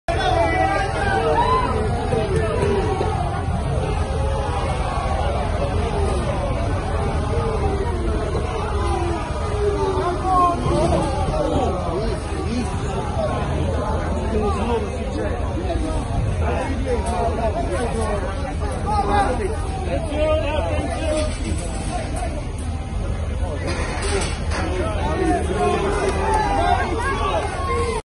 VIDEO IAMNAPLES.IT - Napoli-Juve, l'arrivo all'hotel Parker della squadra bianconera
La squadra allenata da Maurizio Sarri è arrivata a Napoli